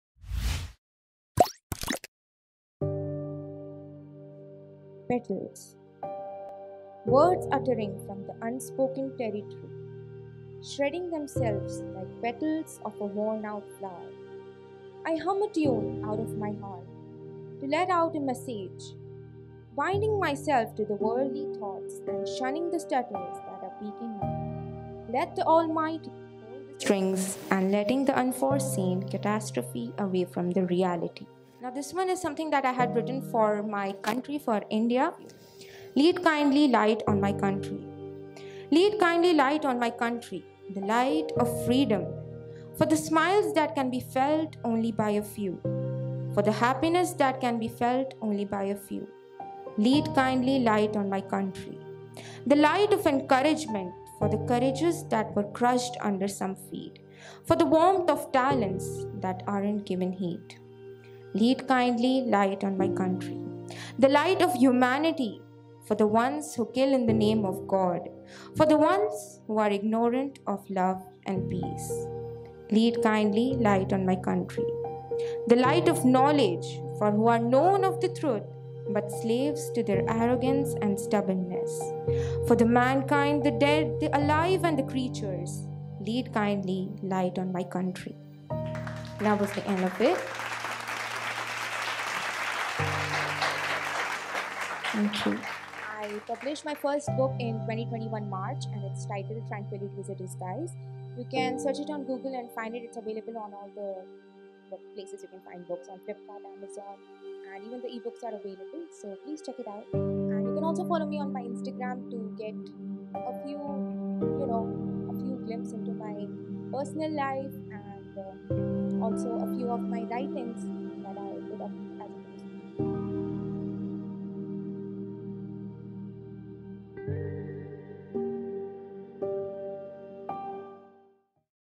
Petals | Lead Kindly Light On My Country by Darakshan Razvi | Inkfeathers Stage Hyderabad
Darakshan Razvi is a young writer and poet who performed two heart-touching pieces that the audience loved at the Inkfeathers Stage Hyderabad. Her first piece titled ‘Petals’ is a small, elegant, simple poem. Her second piece which is just as beautiful addresses India and its people who are a burden to their motherland. Both the pieces she narrated are robust and outstanding. Watch this video until the end to find out for yourself!

Credits:
Written & Performed by: Darakshan Razvi
Event Managed by: Brosnan Antony, Kashish Lewis, Adarsh Kampli, Jessita Fernandez
In association with Heart for Art, Hyderabad
Shot at: Aaromale Cafe & Creative Community, Hyderabad | 16th July 2022